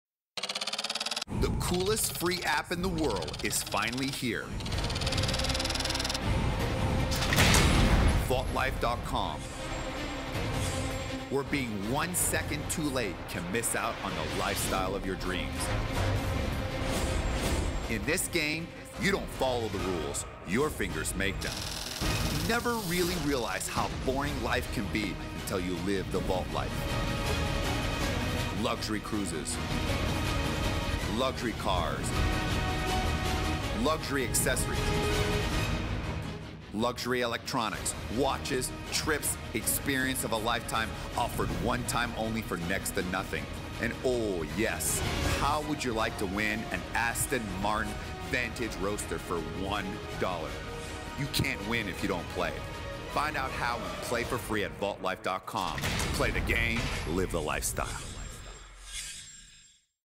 Live Out Loud
A TVC for a online gaming company LiveOutLoud